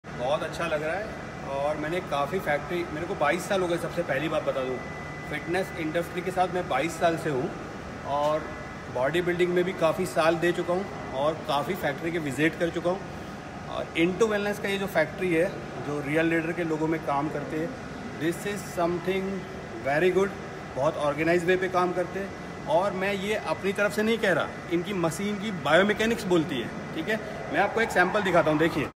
Into Wellness Fitness Equipment Factory Visit & Review by Mr. Ayaz Khalak
Visit of Mr. Ayaz Khalak, a body builder, an industry veteran, an expert in bio-mechanics to our World class fitness equipment manufacturing facility in Pune. We are glad that he liked our systems, processes, quality assurance & way of working in the factory.

Mr. Ayaz Khalak is the founder of VO2+ Fitness in Ahmedabad, Gujarat